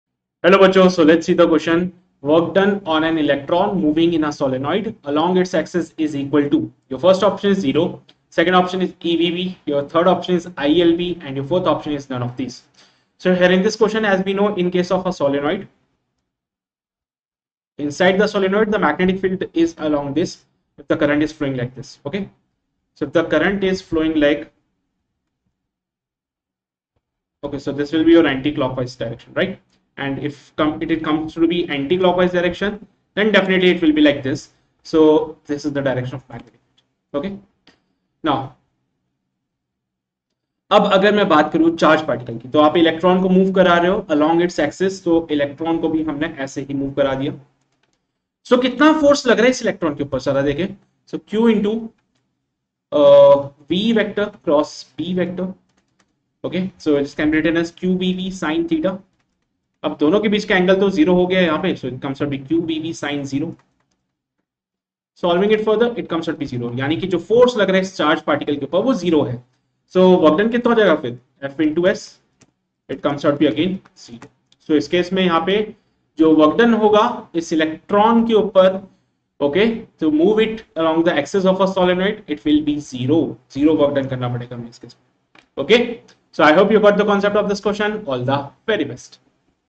Work done on an electron moving in a solenoid along its axis is equal to a. Zero b. evB c. i ℓ B ...
Work done on an electron moving in a solenoid along its axis is equal to
a. Zero
b. evB
c. i ℓ B